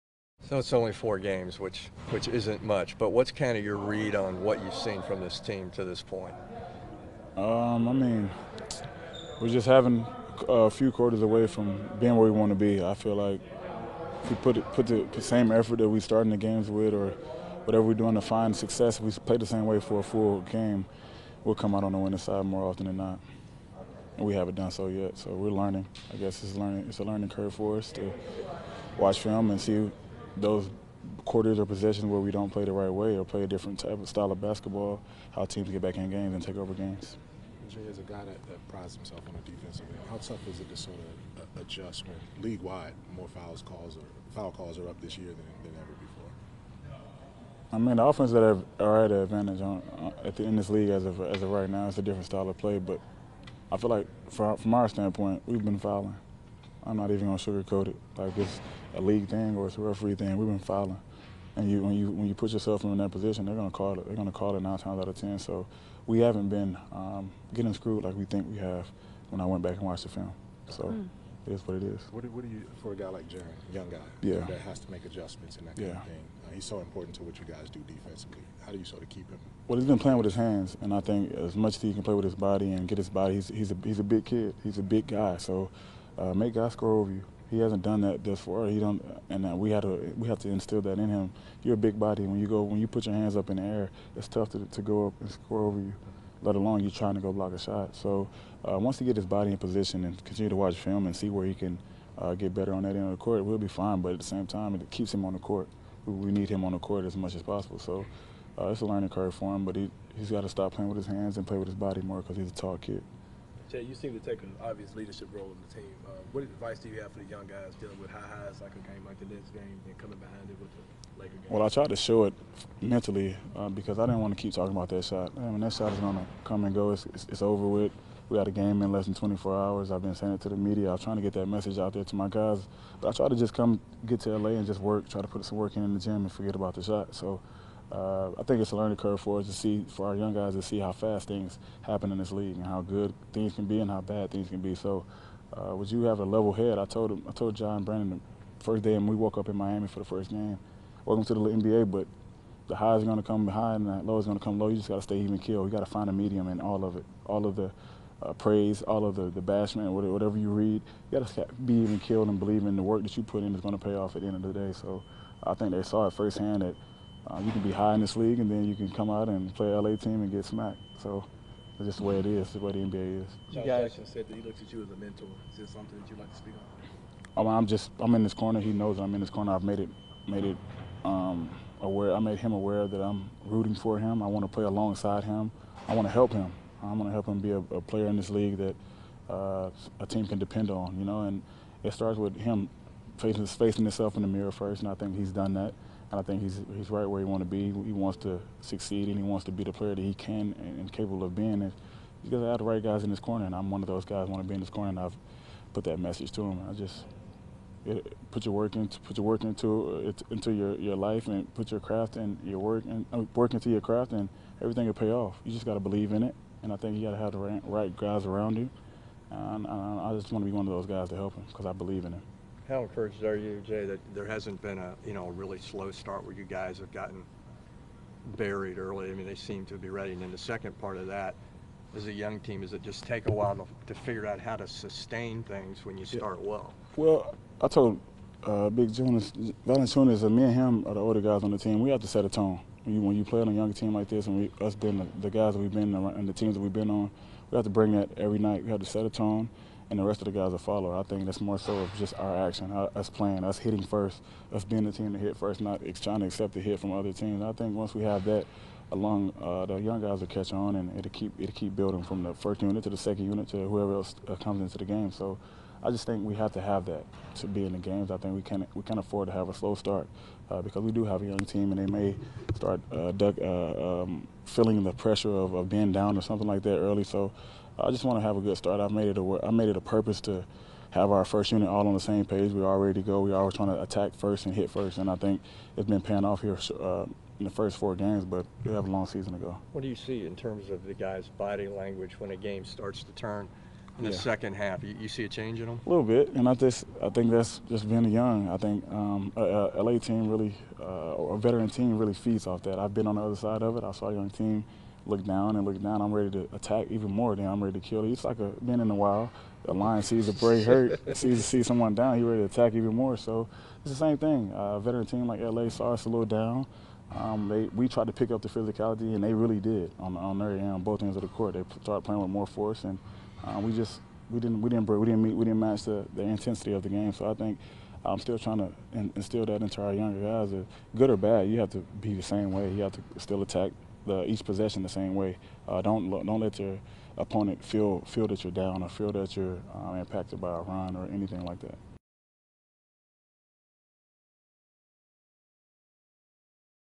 Jae Crowder deals powerful inspiration for rookies: "Put your work into your craft & believe in it"
Forward Jae Crowder talks to members of the media about the team's recent foul trouble, Jaren Jackson Jr.'s development, learning to keep a level head when the good times and the bad times come, and what the rooks need to work on after Thursday's practice at FedExForum.